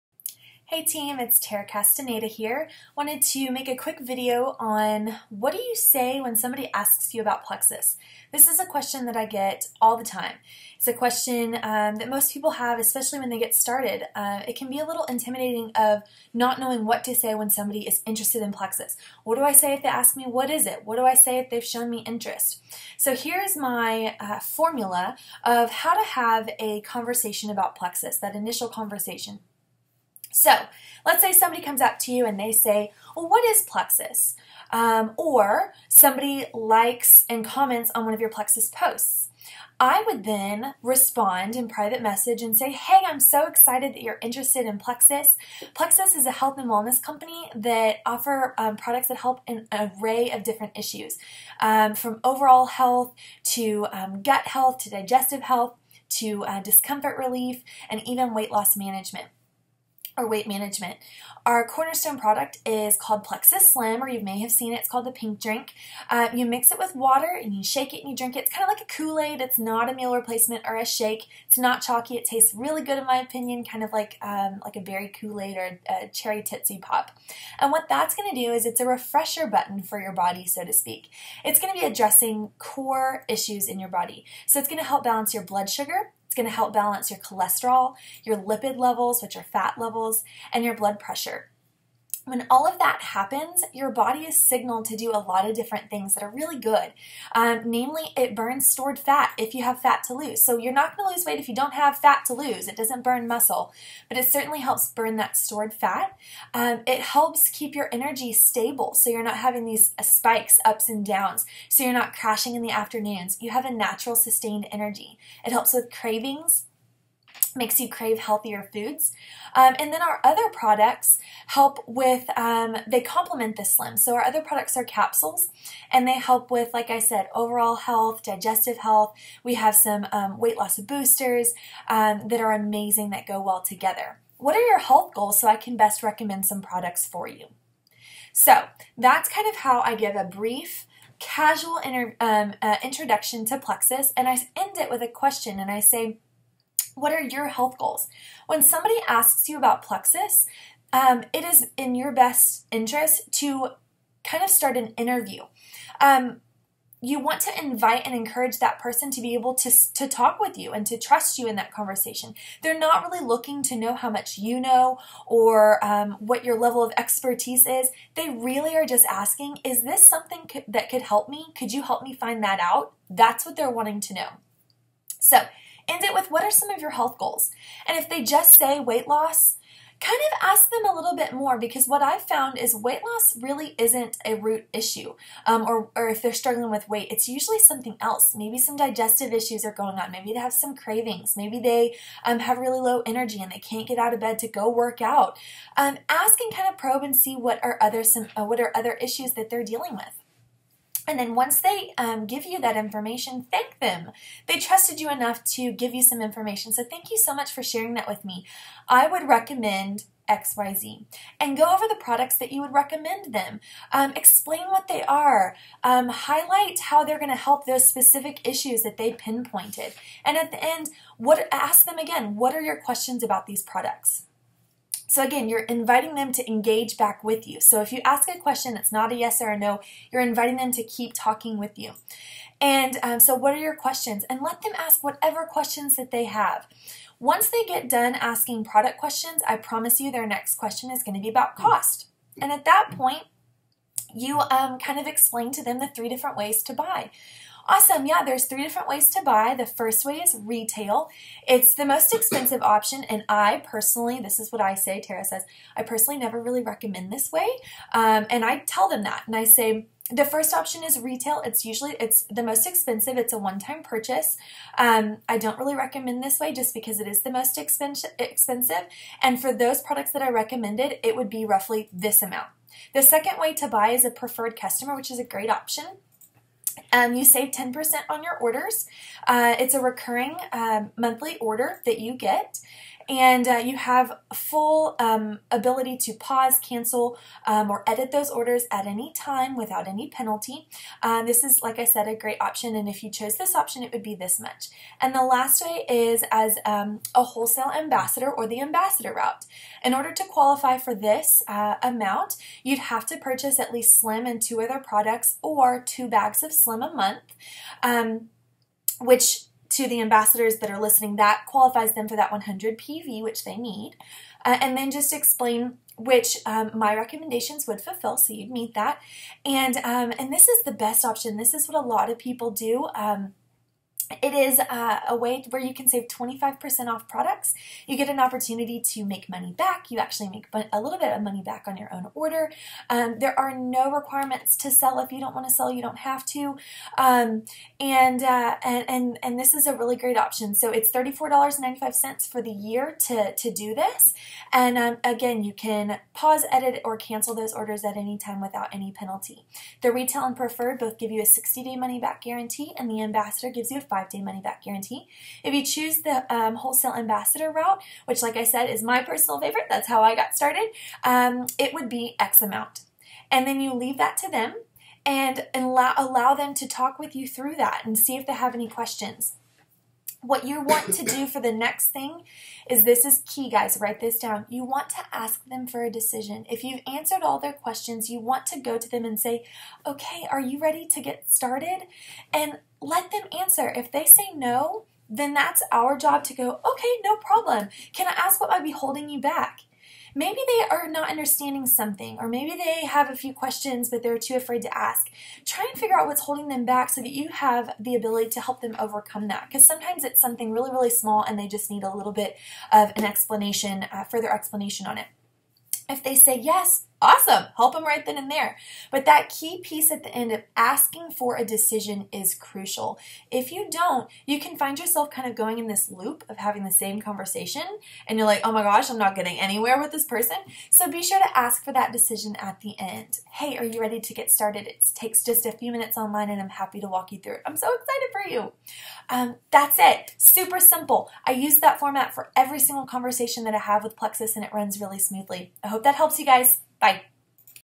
What do I say when someone asks me about PLEXUS?
Hear how I structure an initial conversation about what Plexus is! This is what I use to help gain trust and knowledge to gain new customers!